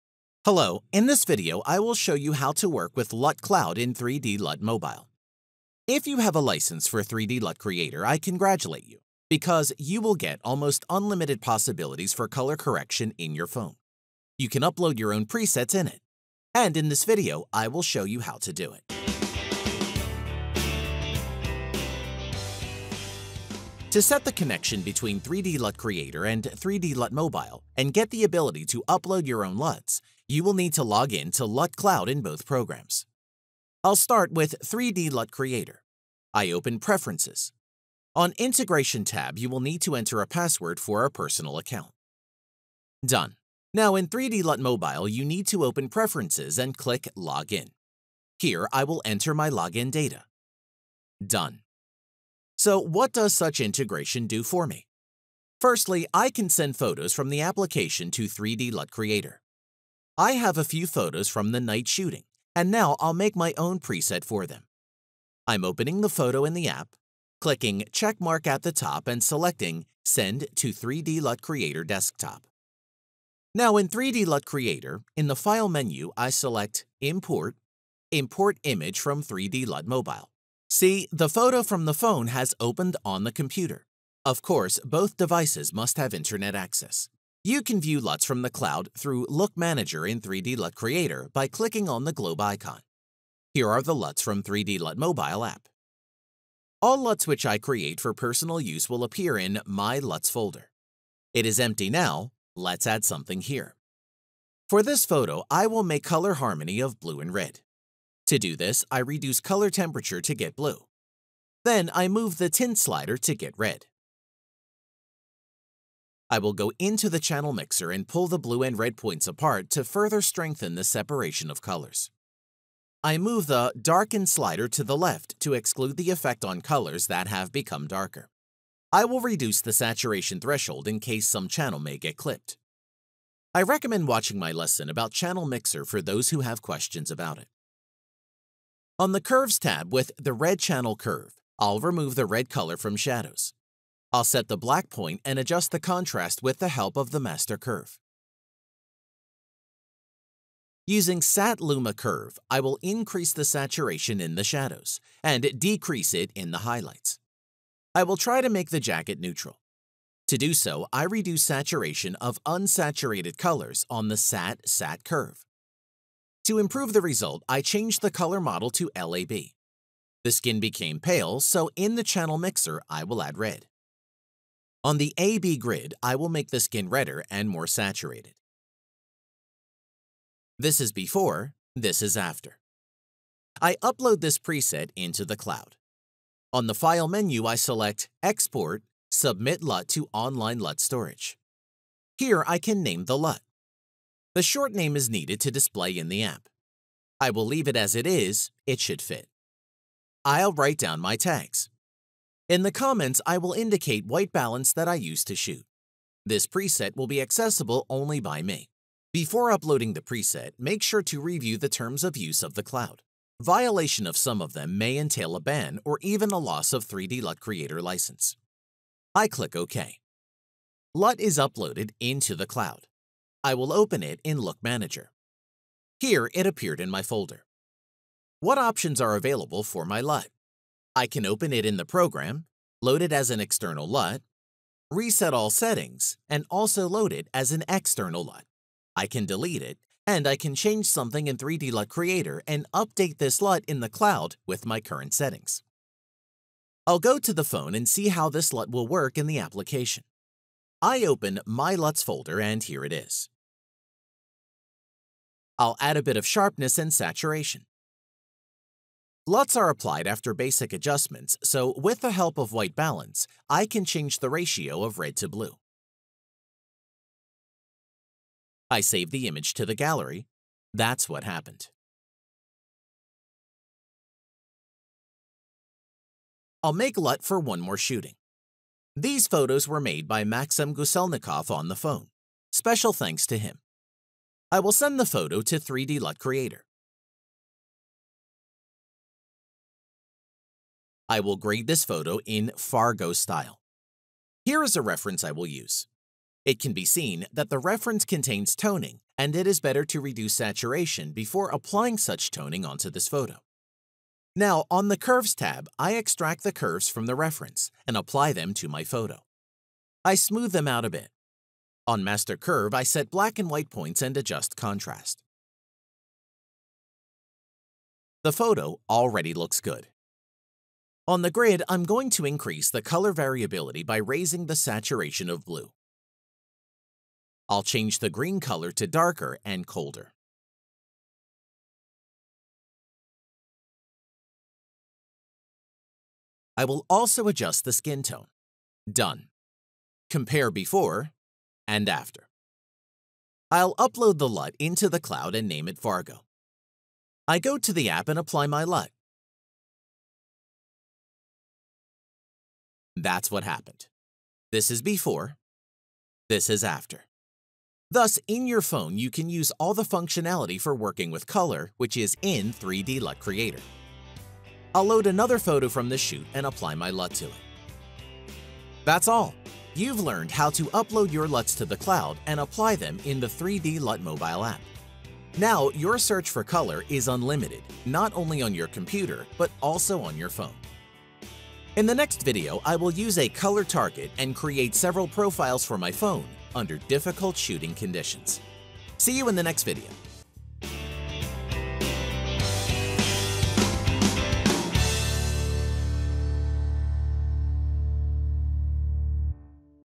LUT Cloud in 3DLUT mobile app, using custom presets in your phone!
In this video I will show you how to work with LUT cloud in 3DLUT mobile. If you have a license for 3D LUT Creator, I congratulate you, because you will get almost unlimited possibilities for color correction in your phone: you can upload your own presets in it! And in this video I will show you how to do it!